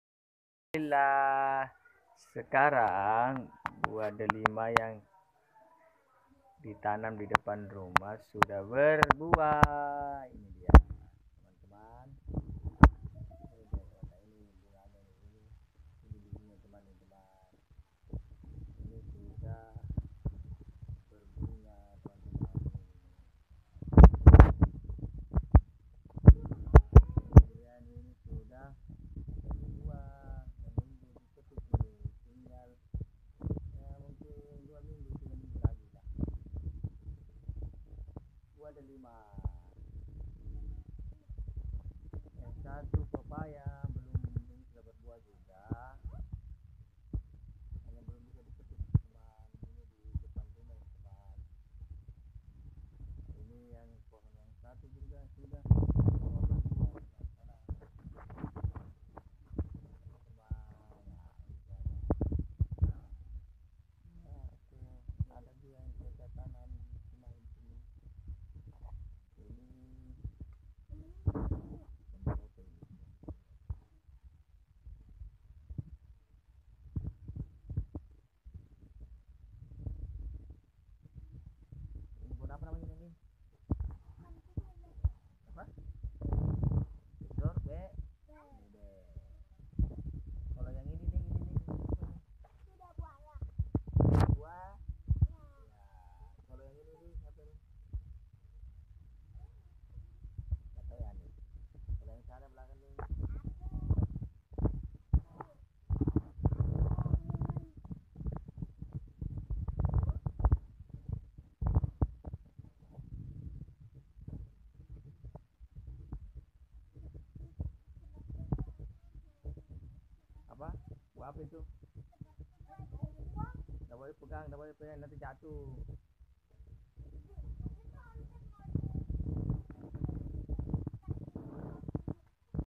Viral Buah Delima yang banyak manfaatnya bagi kesehatan
Pohon buah delima saat ini mungkin jarang ditanam oleh masyarakat bahkan dipasar terkadang sulit dijumpai untuk dijual namun buah delima ini sangat banyak manfaatnya bagi kesehatan dan buahnya sangat bermanfaat bagi kesehatan kita.